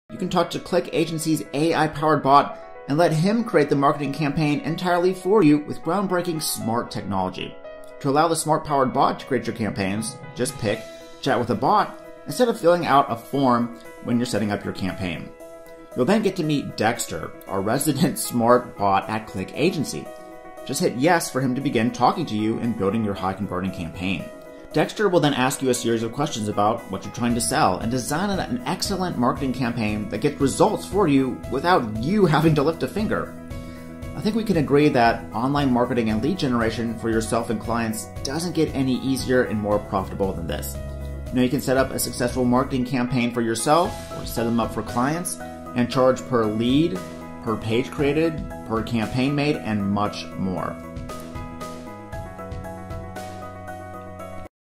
ClickAgency Fully automated software reviewed
The Final ClickAgency Review  Saturday 11th April 2010

NOW IS THIS TOO HARD FOR YOU?

I Have a solution for you. The AI BOT within ClickAgency can do all this smart work for you. All you need to do is follow these shorts steps below

A. Click on "Chat With The Bot".
B. Click Create a Campaign.
C. Add a name for the campaign.
D. Then start to chat and answer questions the BOT has for you. Dexter the Bot will take over and create smart looking pages for you within minutes.

As you can see the online marketing and lead generation is now so simple with just 4 simple steps with Dexter the BOT. You can generate leads for yourself and clients immediately as it doesn't get any easier.

Now you can set up campaign for yourself and clients ad charge per lead, per page created per campaign.

I hope you enjoyed the review and comprehensive study of ClickAgency I have done for you so that you can make an informed decision if this software suits you.

Wishing you all the best in marketing and getting more leads with ClickAgency.